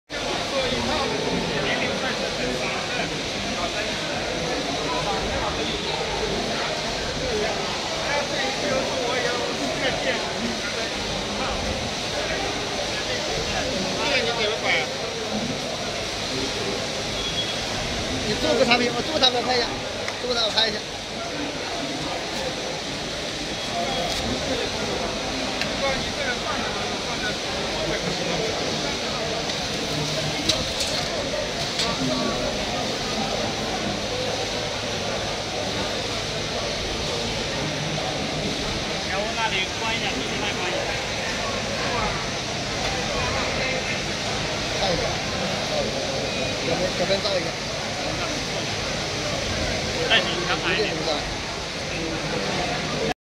Full-auto Shrinking Film Sealing Machine
一、General Description
The machine specially used for machine,diesel rotary filter in and out of the oil sealing mouth heat shrinkable film.
二、Specification
1、Seal speed:40pcs/min
2、Filter diameter range:Ф60-Ф110mm
3、Filter height:60m-140m
4、Motor power:8kw
5、Working air pressure:0.6Mpa
6、Power supply:380v/50Hz
7、M/C weight:500kg
8、M/C dimension:2600×1485×1800(l×w×h)
三、Features
1、this machine adopts the cams composite,which is running stably and precisely location.
2、this full automatic sealing machine has the automatic circulation of the shrinking film edge cutting,filter loading in arm robot and curing oven.